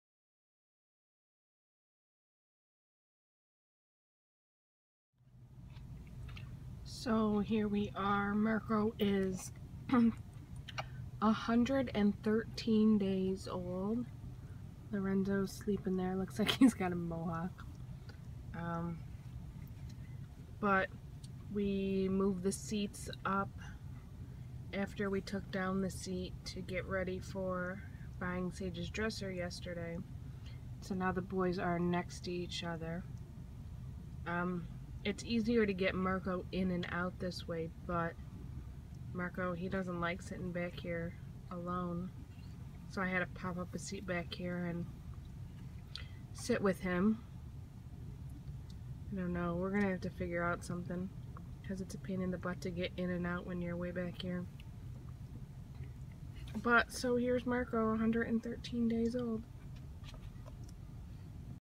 Marco 113 Days Old
We moved Marco up in the minivan. He's not liking sitting somewhere where his mom or dad or even Sage isn't sitting next to him.